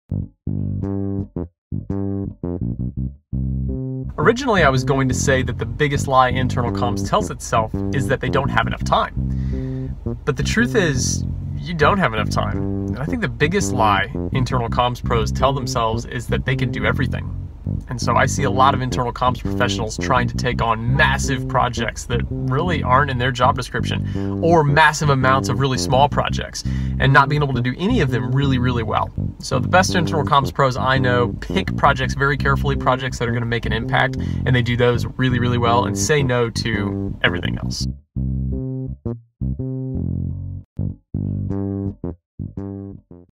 Something Else - November - Rocky Walls
What is the biggest lie internal communications tells itself? Watch Rocky Walls'' answer.

"Originally, I was going to say that the biggest lie internal comms tells itself is that they don't have enough time. The truth is you don't have enough time, and I think the biggest lie internal comms pros tell themselves is that they can do everything, and so I see a lot of internal comms professionals trying to take on massive projects that really aren't in their job description or massive amounts of really small projects and not being able to do any of them really, really well. The best internal comms pros I know pick projects very carefully, projects that are going to make an impact and they do those really, really well, and say no to everything else."